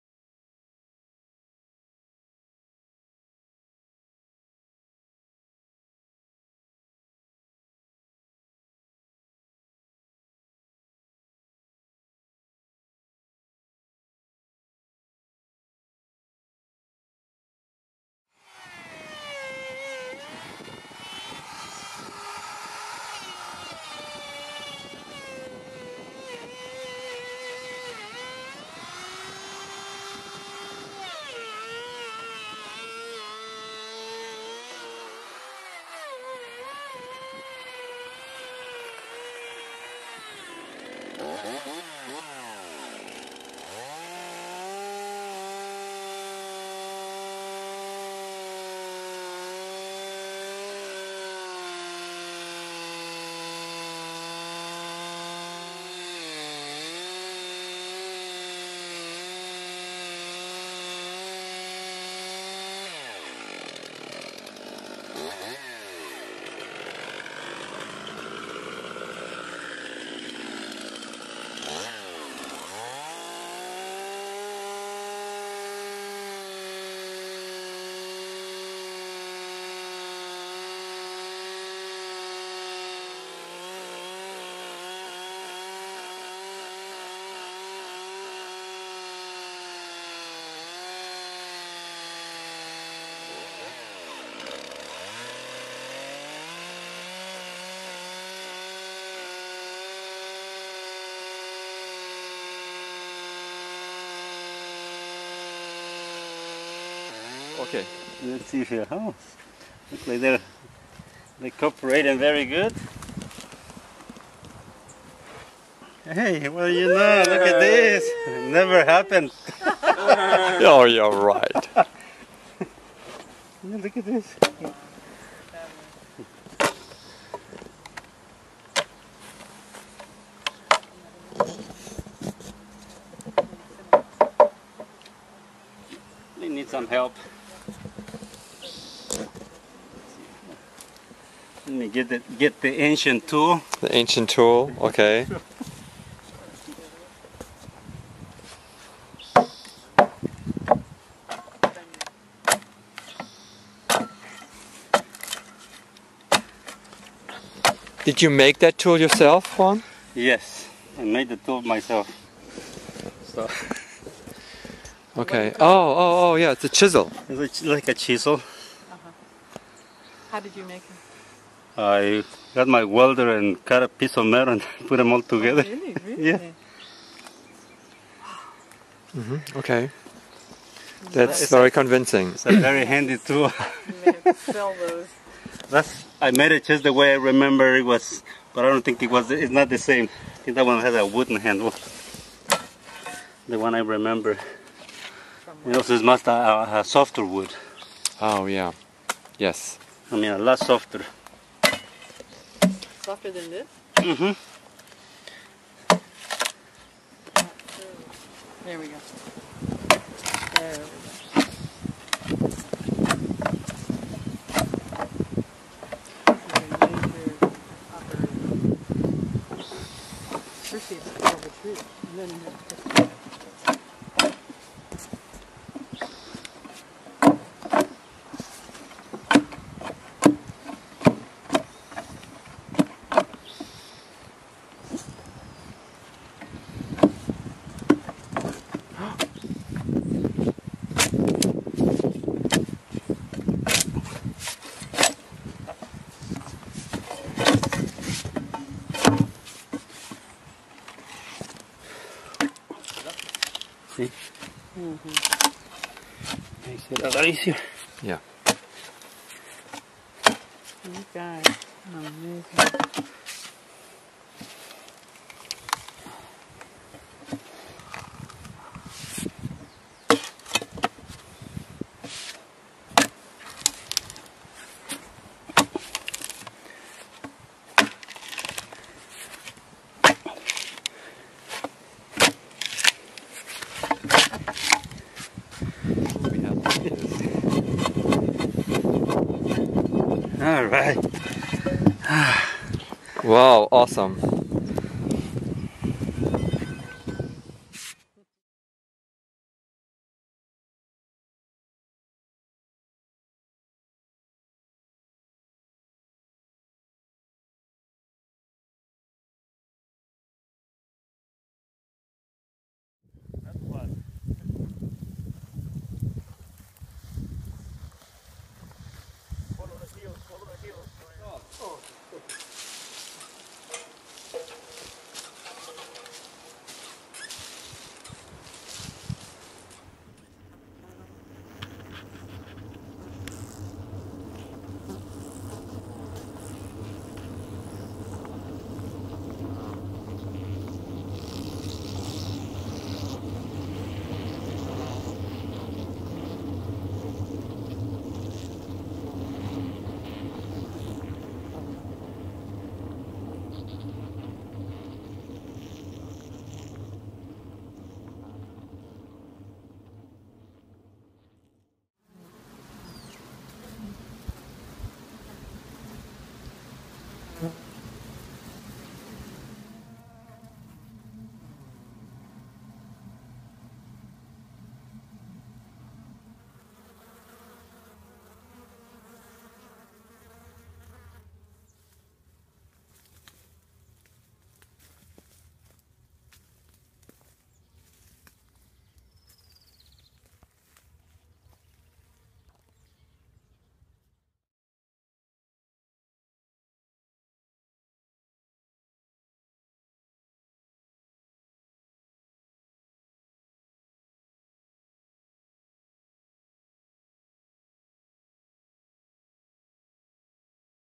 The Art of Log Hive Making
This video is about the art of Log hive making. Log hives provide a natural nest environment and allow bees to grow their one-body according to their instincts and inner wisdom.  Frameless nest environments support health and emotional well being.